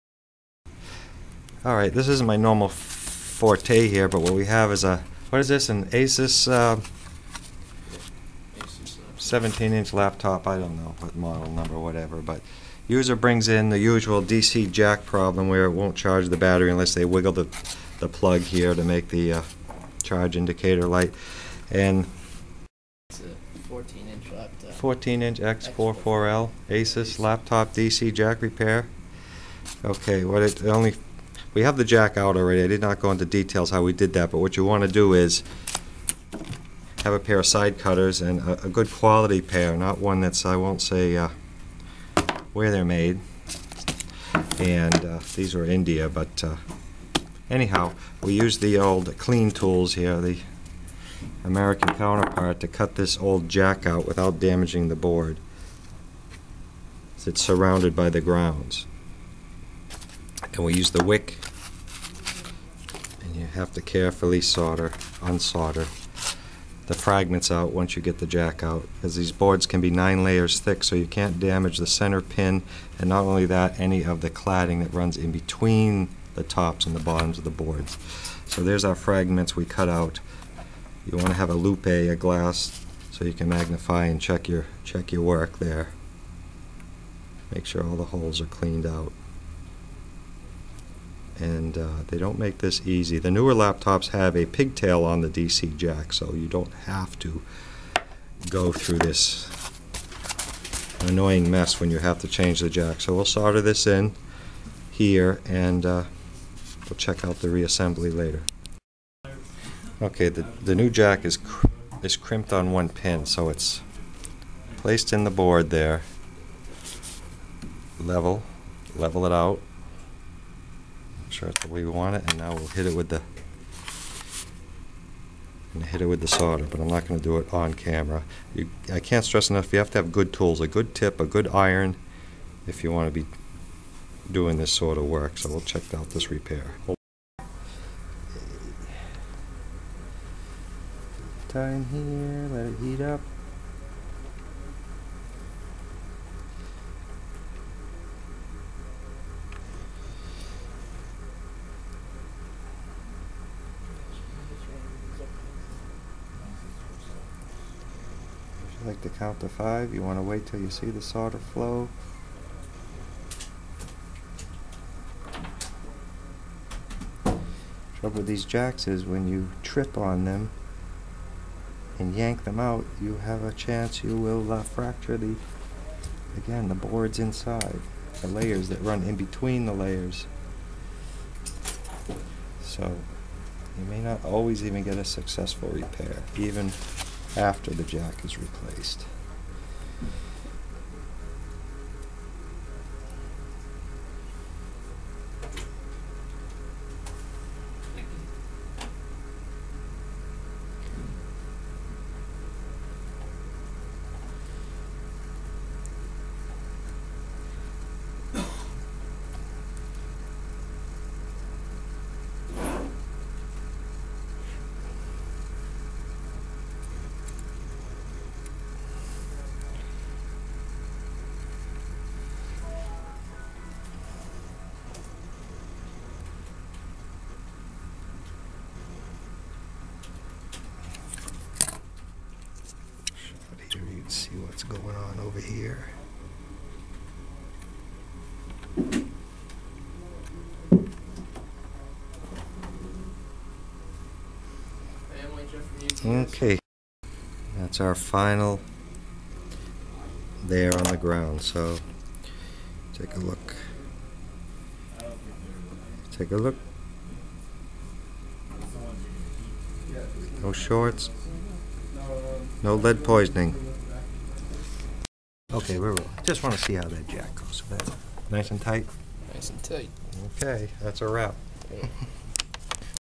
Asus X44L DC Jack Repair By Replacement.
A quick overview on the procedure for the successful removal/replacement of the DC charging jack. You need good soldering skills, good soldering station, a good tip, solder braid, good side cutters, (to cut the old jack out), & a good magnifier to check your work. Then it will ready to be able to accept the replacement jack. The multi-layer boards used in these devices use plated through holes, and may be upwards of 9 layers thick. the plating on these holes must remain in good condition. Sometimes, if the jack was physically pulled hard these boards can be fractured in the jack area & may not work properly even with a new jack installed. That was not the case here. Most newer laptops have the jack on a pigtail connector, so no soldering is required. We 1st. "mended" the solder connections, but the jack itself still had a poor connection, so it was replaced for a more permanent repair.